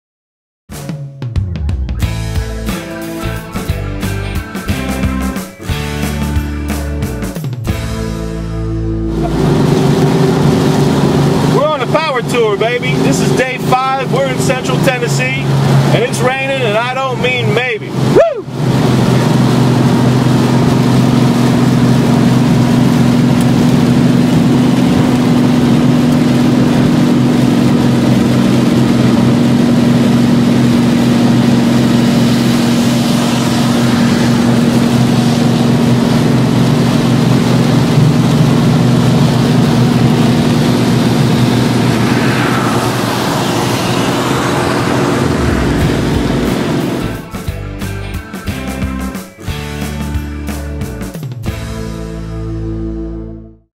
Pioneer @ Hot Rod Power Tour 2010: A Bit of Weather
Heavy rains on Day 5 of the 2010 Hot Rod Power Tour. Take a ride with Harry Kroll as he drives Pioneer's 1969 Chevy Camaro to Chattanooga, Tennessee.